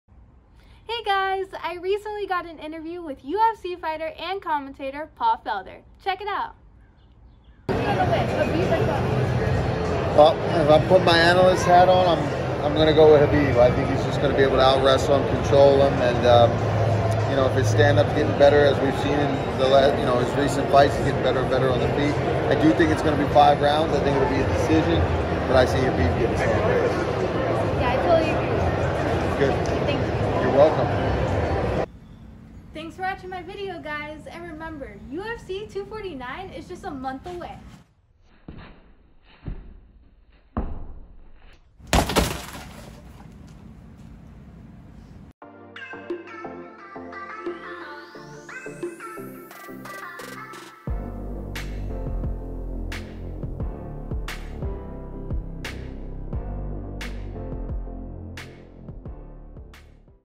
Mini Khabib interviews Paul Felder about ufc 249 khabib vs tony
Hey guys Paul felder was nice enough to let me interview him about ufc 249 and his thoughts on khabib vs tony